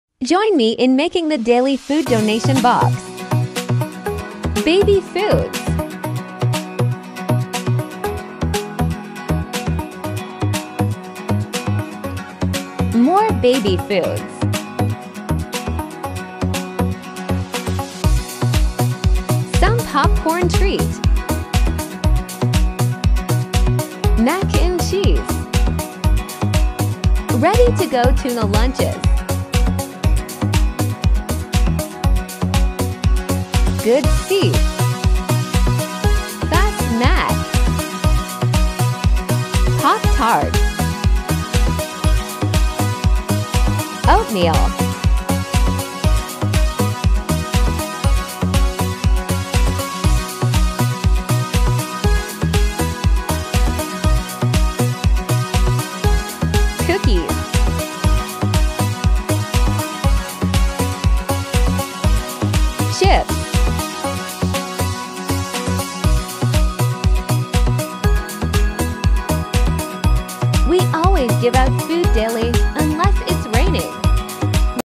making the daily food donation box (11.11.2021) [TikTok]
we have such an over load of food in our house thanks to pandemic that we couldn't keep it in our small apartment. so we are doing a great thing since September is putting out a food donation box for the city. it's food people who need a quick meal or just food for the day. its an awesome thing to do for helping your community out!